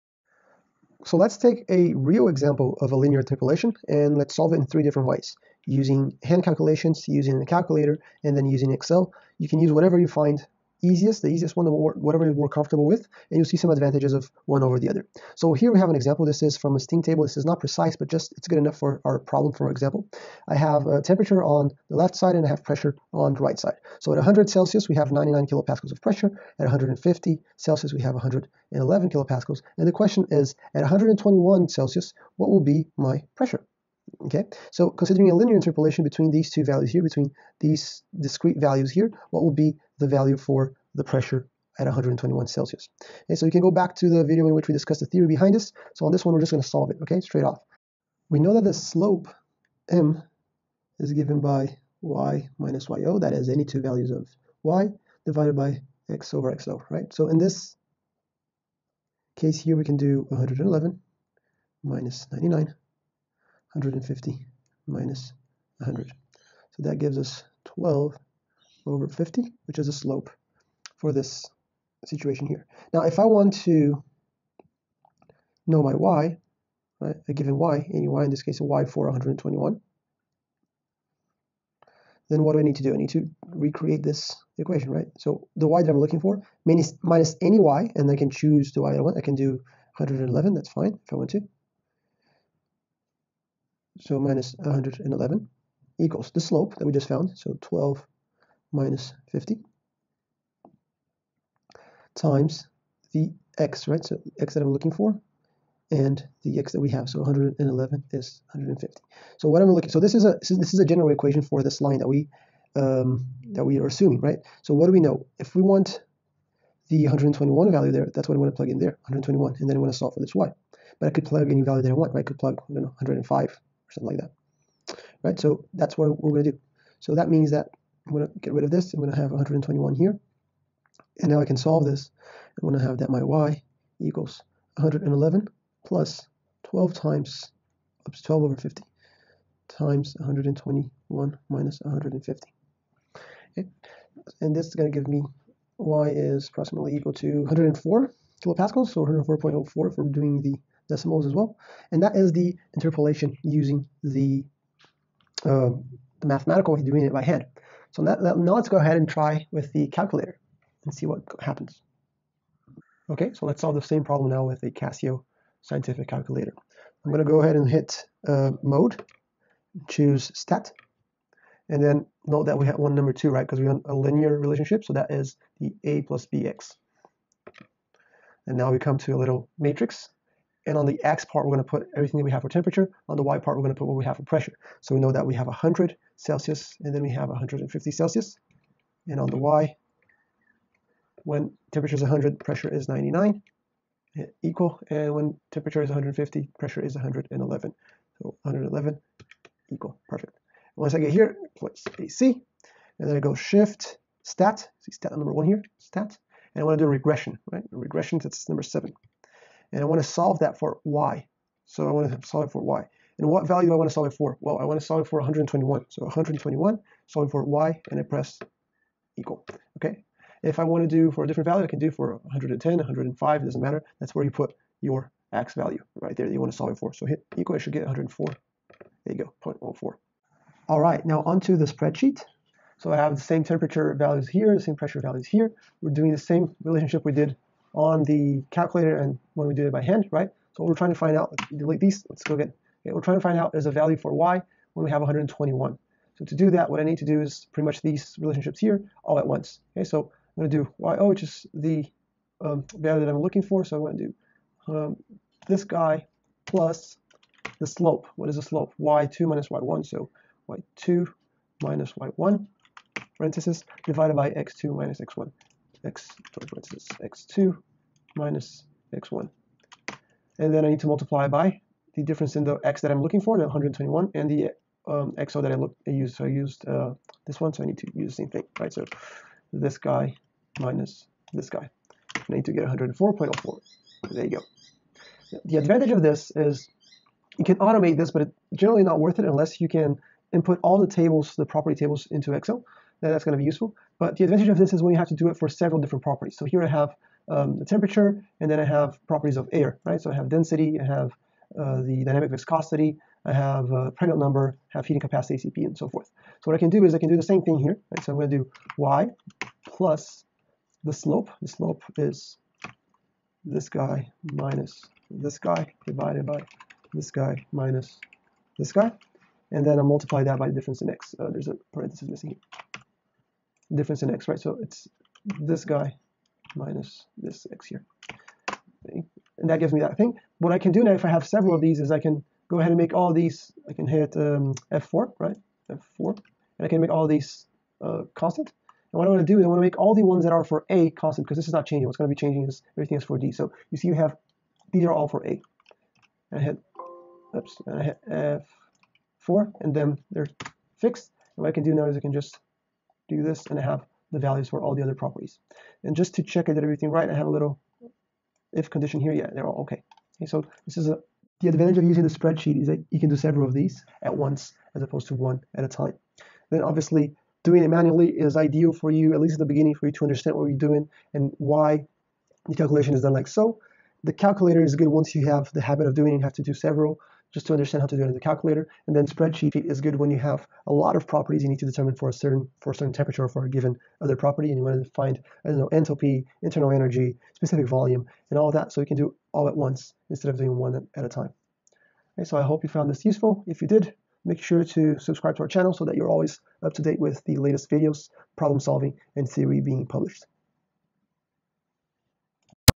3 Ways to do a Linear Interpolation | Paper | Calculator | Spreadsheet
00:00 Intro
00:20 Problem Statement
00:48 First Way - Calculating by hand
02:43 Second Way - Scientific Calculator
03:50 Third Way - Using a spreadsheet
06:06 Advantages of each method

Welcome to Engineering Hack! In today's video we are talking about linear interpolation. But instead of just talking about the theory, we are actually solving a linear interpolation for a steam table example in 3 different ways: doing it by hand (pen and paper), doing it with a scientific calculator and doing it on a spreadsheet. They all have their advantages over the other methods, and we''ll briefly talk about them. But in short: you want to be sure to do it by hand as many times as you need to understand what you are doing and why you are doing it. Once it makes sense to you, you don't have to do it by hand anymore (unless you are a student and class requires you to do it by hand - its a bit old fashion, but I am sure some schools still ask for this). Once you have mastered the idea behind it, you are welcome to always use a calculator and to be proficient on using it. Especially because when dealing with problems of thermodynamics or heat & mass transfer you'll need to do this all the time. But if you always have a spreadsheet handy because you're always carrying a laptop computer or a tablet with you, then using the spreadsheet can end up saving you time, as you can do several interpolation (for several properties) at once.

Problem Statement: Given the property table with information for temperature and pressure, find the pressure corresponding to 121°C using linear interpolation.

To learn more please watch the video till end and if you have a question just ask in the comments section.